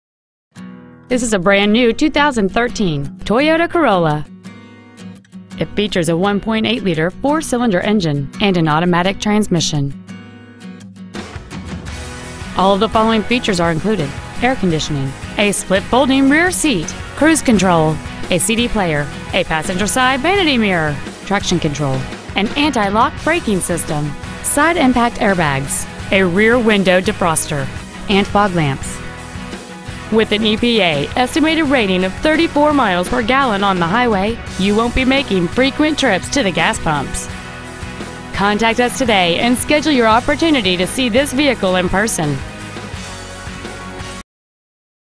2013 Toyota Corolla Concord CA 94520
Year : 2013
 Make : Toyota
 Model : Corolla
 Engine : 1.8L I-4 cyl
 Trans . : Automatic
 Exterior : Tropical Sea Metallic
 Miles : 1
 Interior : Dark Charcoal
 Concord Toyota
 2013 Toyota Corolla Concord CA